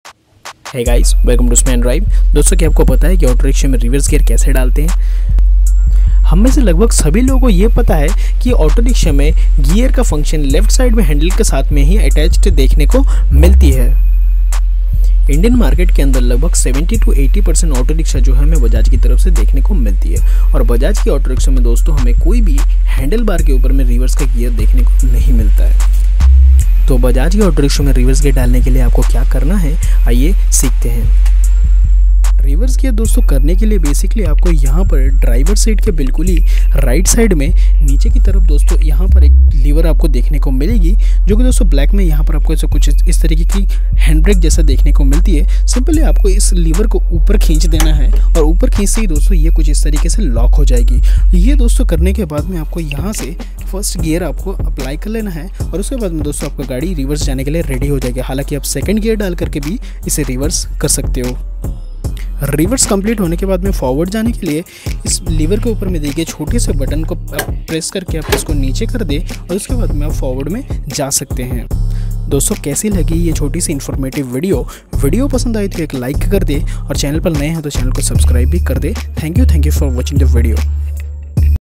Autorickshaw Reverse Kaise Kare | Sman Drive Vlogs | Bajaj Autorickshaw
this video is about.. how to put autorickshaw in reverse gear..

basically how to put reverse gear in Bajaj autorickshaw

Dosto.. simply apko driver seat ke right me niche diye gaye gear ko pull kar ke upar lock kar Dena hai.. or fir hath me diye gear ko 1st ya 2nd me dalke reverse karna hai...

reverse complete hone ke baad simply app..same gear ke upar diye gaye button ko press karke use down kar Dena hai.. or fir app forward (aage) ja sakte hai

dosto ummid hai ke video apko pasand ayegi..